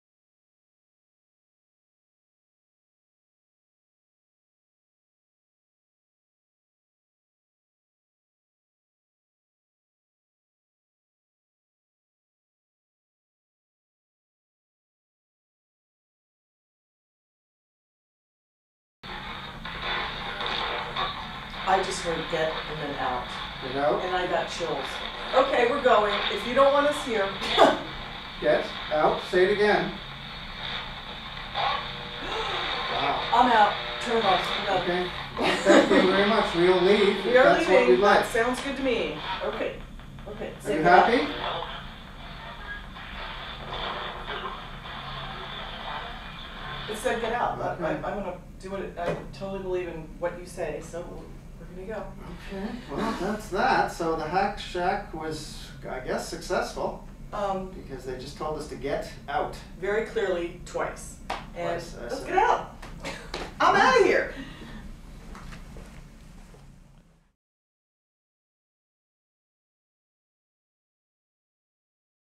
Haunted Texas - Driskill Hotel Shack Hack with Spirit Light
On Wednesday, July 6th, 2011 during a paranormal investigation at The Driskill Hotel in Austin, Texas, 'Haunted Texas' conducted a "Shack Hack Ghost Box" session in Room 430 at approximately 10:30pm with some very startling results...